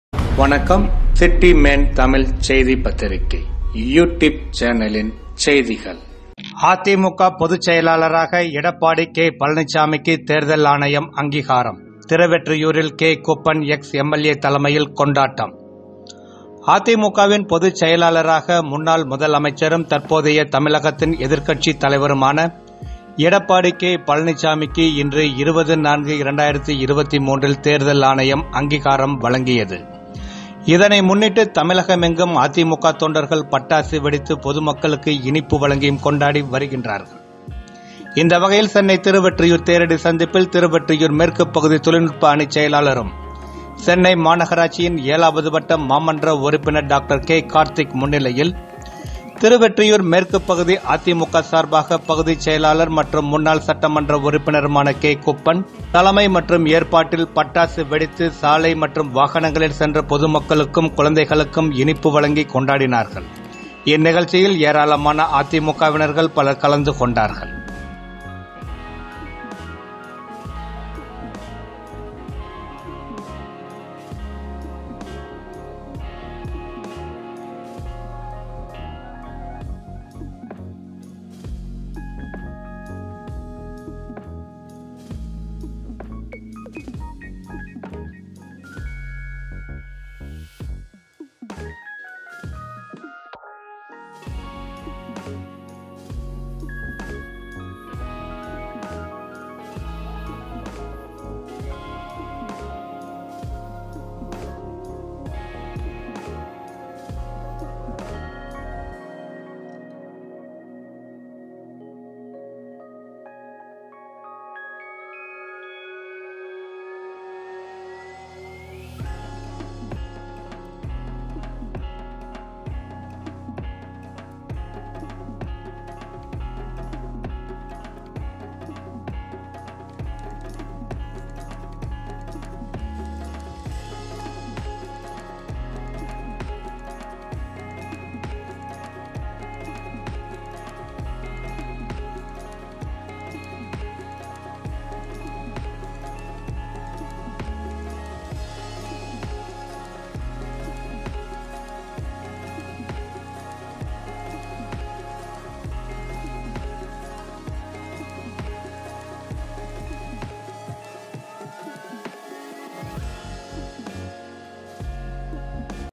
அதிமுக பொதுச்செயலாளர் EPSக்கு தேர்தல் ஆணையம் அங்கீகாரம் K.குப்பன் தலைமையில் கொண்டாட்டம் 20.4.23
அதிமுக பொதுச்செயலாளராக  எடப்பாடி கே பழனிச்சாமிக்கு தேர்தல் ஆணையம் அங்கீகாரம் திருவொற்றியூரில் கே குப்பன் Ex.MLA தலைமையில் கொண்டாட்டம் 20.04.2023
அதிமுகவின் பொதுச்செயலாளராக முன்னாள் முதலமைச்சரும் தற்போதைய தமிழகத்தின் எதிர்க்கட்சித் தலைவருமான எடப்பாடி கே பழனிச்சாமிக்கு இன்று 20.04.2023 ல் தேர்தல் ஆணையம் அங்கீகாரம் வழங்கியது. இதனை முன்னிட்டு தமிழகமெங்கும் அதிமுக தொண்டர்கள் பட்டாசு வெடித்து, பொதுமக்களுக்கு இனிப்பு வழங்கியும் கொண்டாடி வருகின்றார்கள். இந்த வகையில் சென்னை திருவொற்றியூர் தேரடி சந்திப்பில் திருவொற்றியூர் மேற்கு பகுதி தொழில்நுட்ப அணி செயலாளரும் சென்னை மாநகராட்சியின் 7வது வட்ட மாமன்ற உறுப்பினர் டாக்டர் கே கார்த்திக் முன்னிலையில் திருவொற்றியூர் மேற்கு பகுதி அதிமுக சார்பாக பகுதி செயலாளர் மற்றும் முன்னாள் சட்டமன்ற உறுப்பினருமான கே. குப்பன் தலைமை மற்றும் ஏற்பாட்டில் பட்டாசு வெடித்து சாலை மற்றும் வாகனங்களில் சென்ற பொதுமக்களுக்கும் குழந்தைகளுக்கும் இனிப்பு வழங்கி கொண்டாடினார்கள் இந்நிகழ்ச்சியில் ஏராளமான அதிமுகவினர் பலர் கலந்து கொண்டார்கள்.